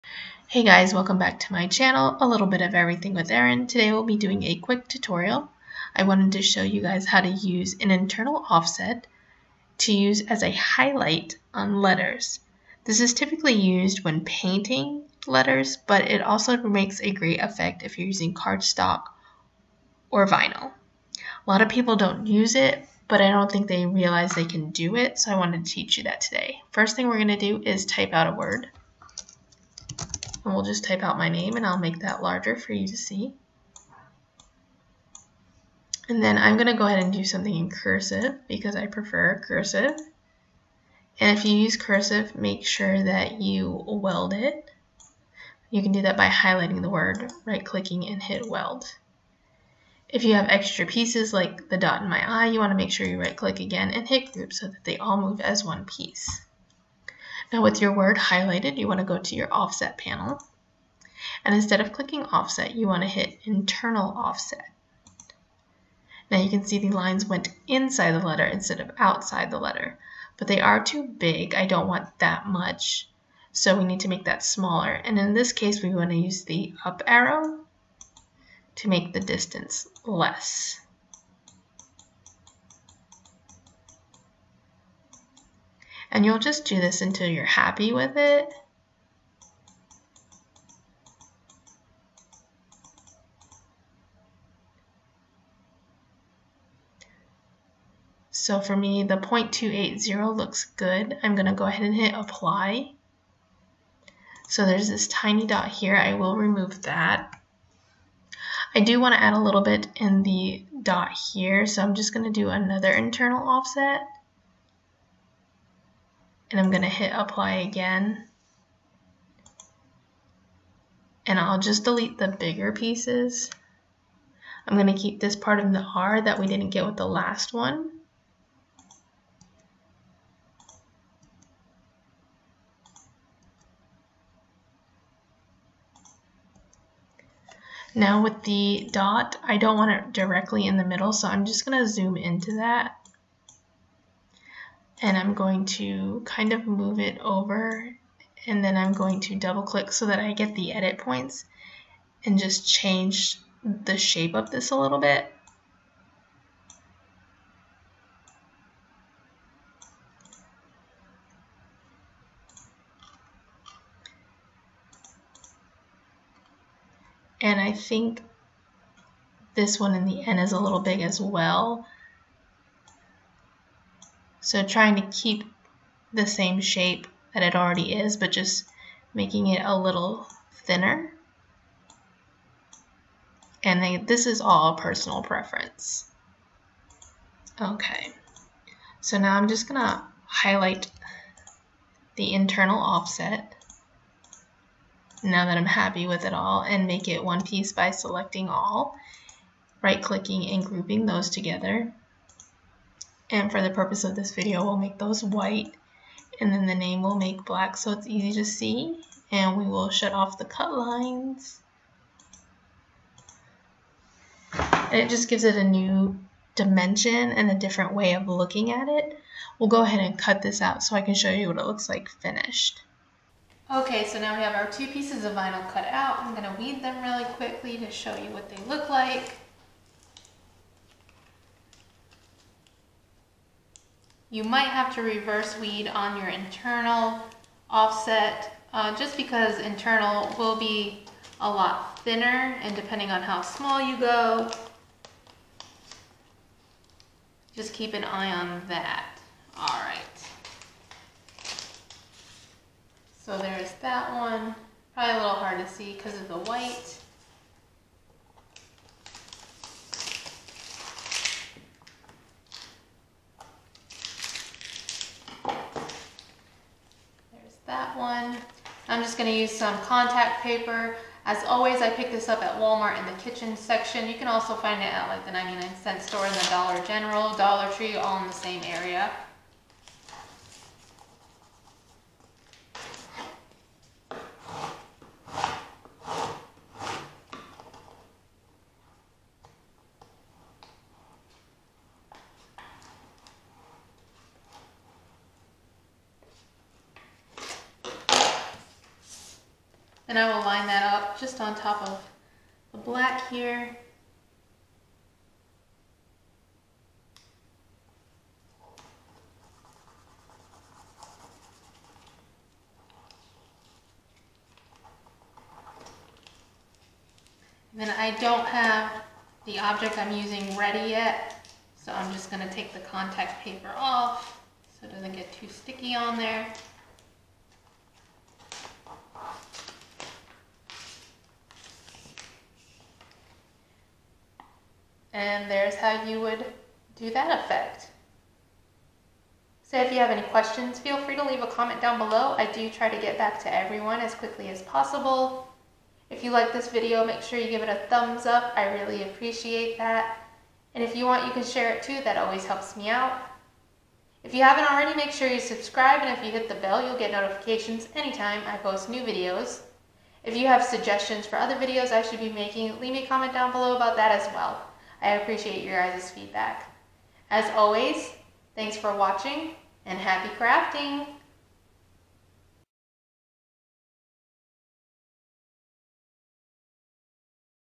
Adding An Internal Offset To A Word To Create A Highlight
Hey guys! Quick tutorial on how to add a highlight by using the internal offset function.

Erin